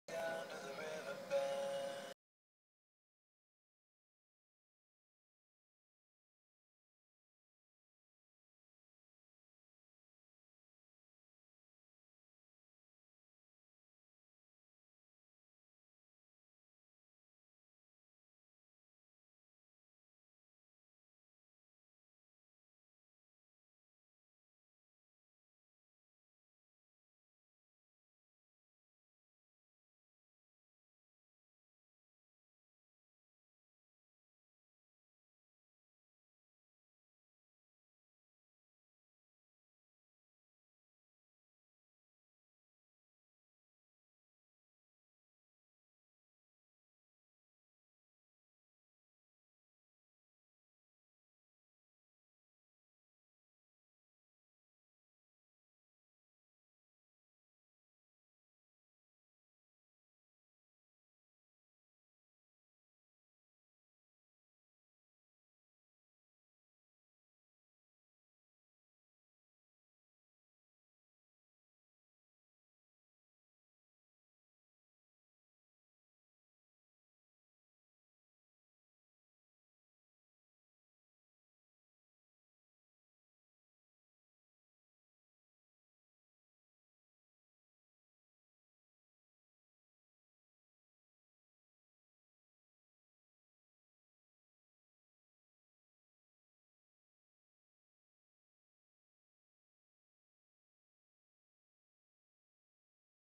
Morphos Vs AmigaOs4.1 Video youtube
MacMini G4 1.5 Ghz 1gb Morphos 3.3 System resolution 1280x1024
Video H264 Accelerated 360p thru Owb browser Ipad Spoofing
Realtime Play